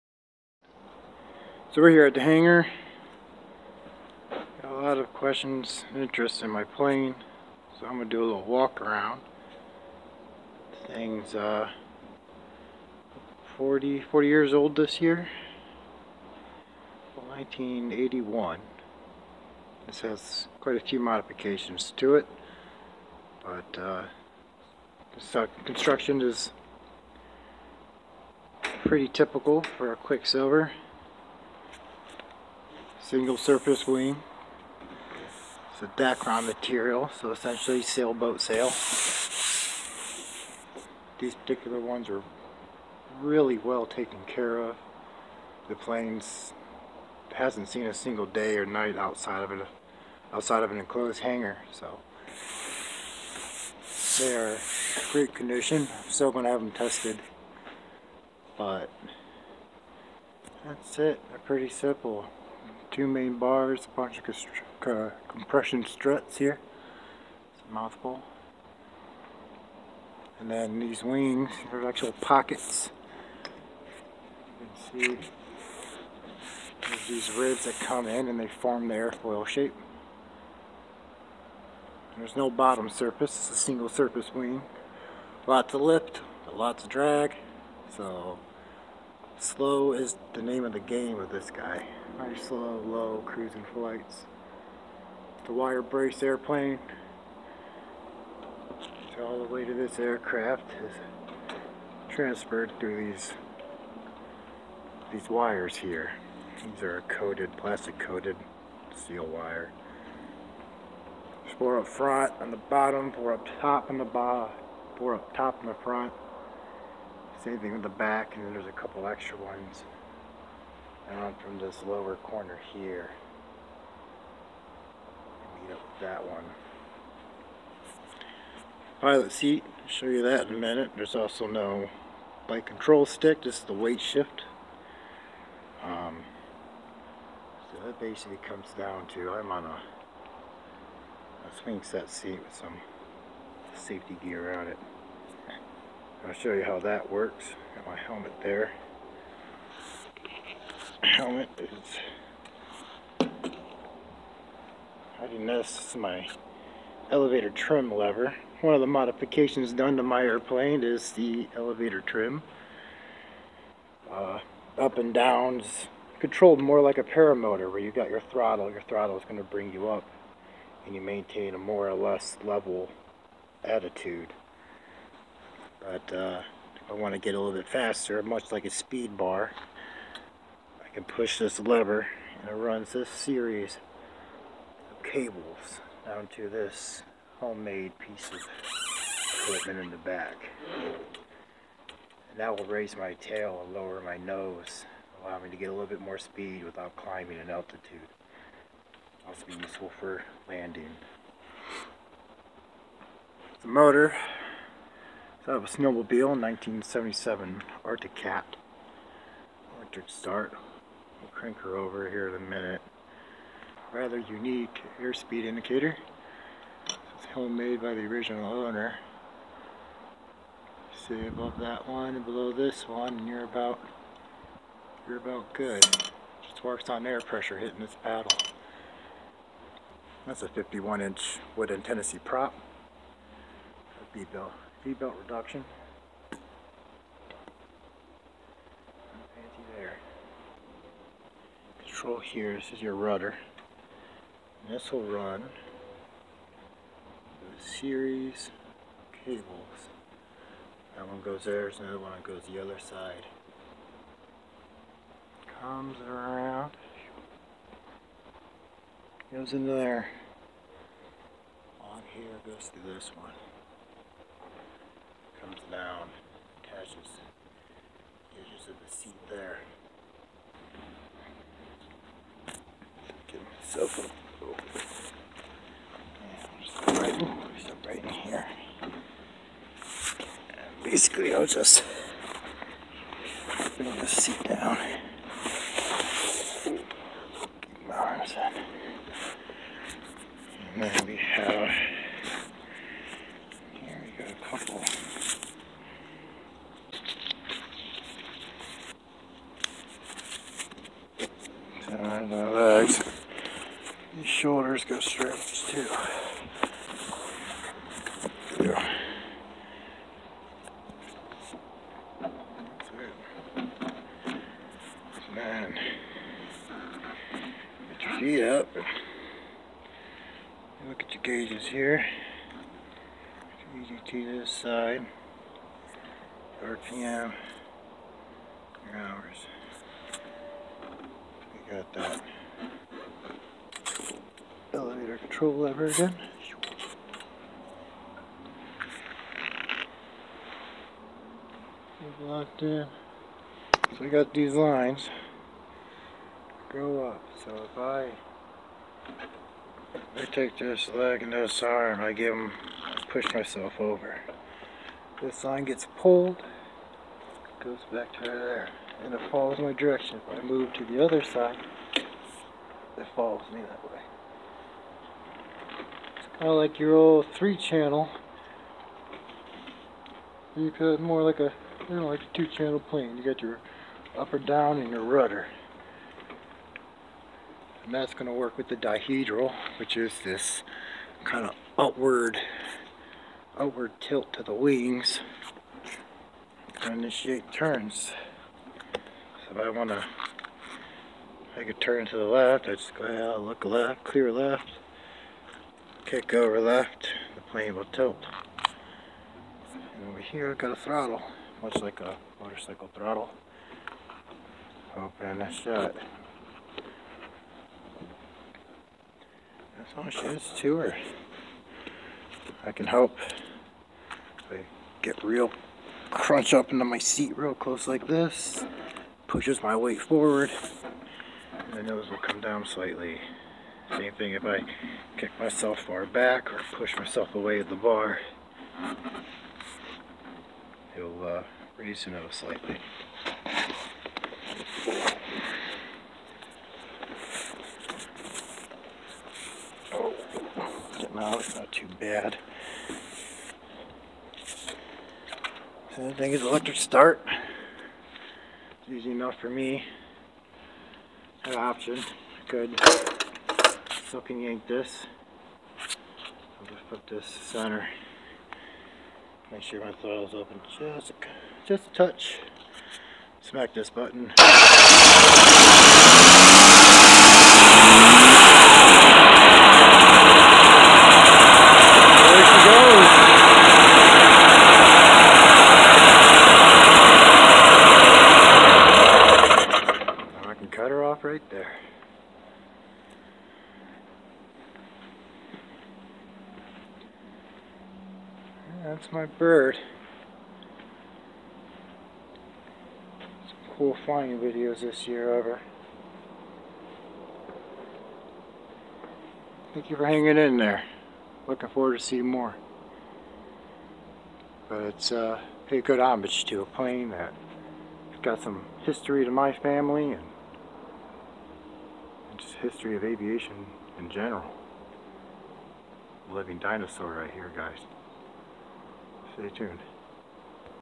Prop Life Premier: 1981 Quicksilver Eipper, Weightshift Ultralight Walk-around
Join me as I take a walk around my 1981 quicksilver weightshift ultralight in this channels first video.